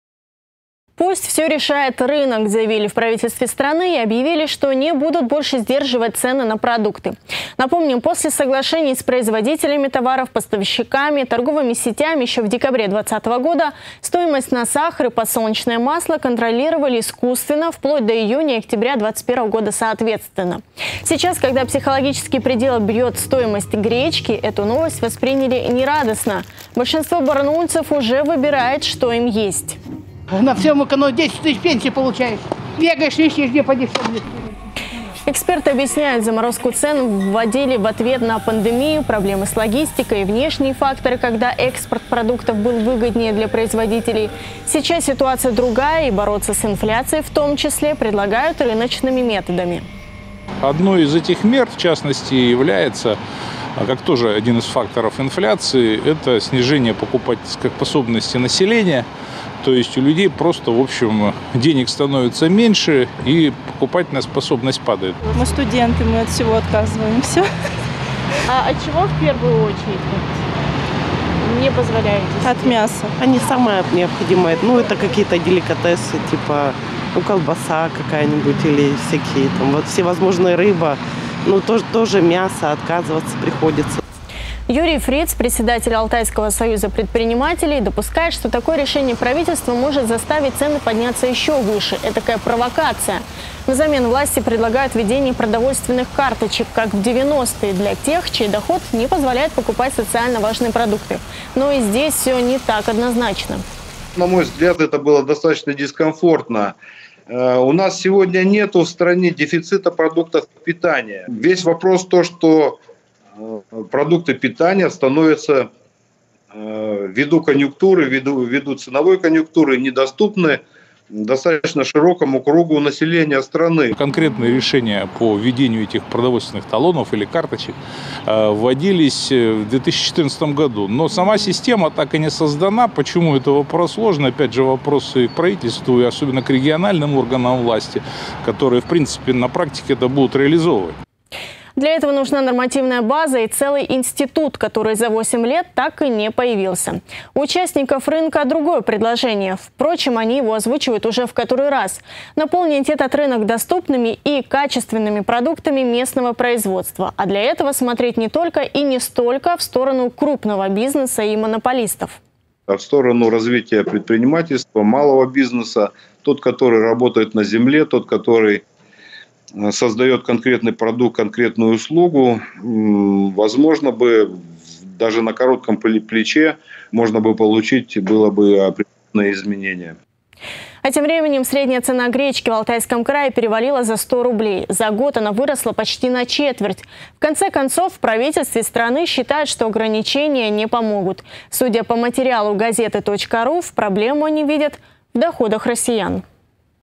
Российское правительство больше не будет сдерживать цены на сахар и подсолнечное масло
Российское правительство больше не будет сдерживать цены на популярные продукты пищевой корзины – сахар и подсолнечное масло. Власти решили отойти от практики соглашений с бизнесом и торговыми сетями и делать ставку на рыночные механизмы. И, возможно, даже возродить продовольственные талоны для тех, кто мало зарабатывает